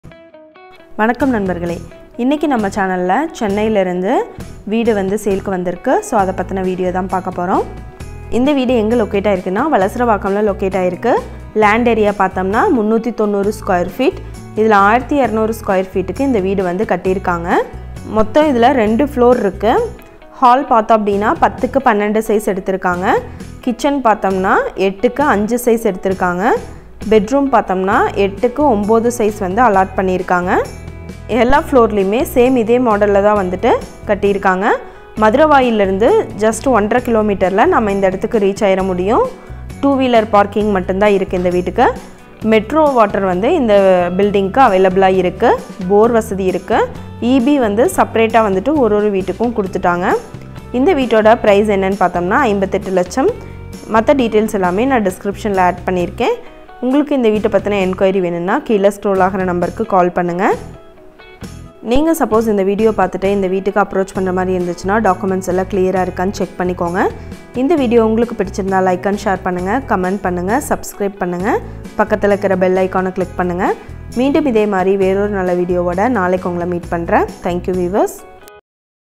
House For Sale/Shree Sankavi Properties/Tamilnadu house for Sale/390 sqft
Chennai
Valasaravakkam
madhuravoyal 1 1/2 kms
Ground floor
first floor.
Second floor
Hall 10*12
kitchen 8*5
bedroom 8*9

2 wheeler parking only
borewell
metro water
eb seperate

Price
58 lakhs